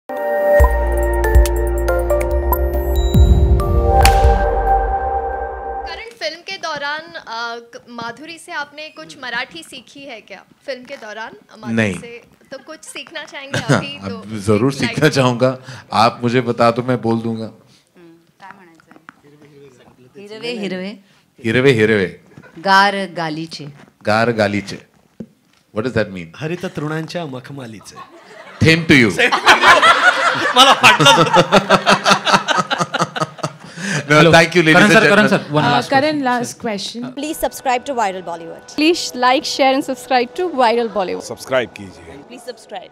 Madhuri Dixit Teaching Karan Johar Marathi | FUNNY MOMENT | Karan Johar ki Marathi
WATCH Madhuri Dixit Teaching Karan Johar Marathi | FUNNY MOMENT | Karan Johar ki Marathi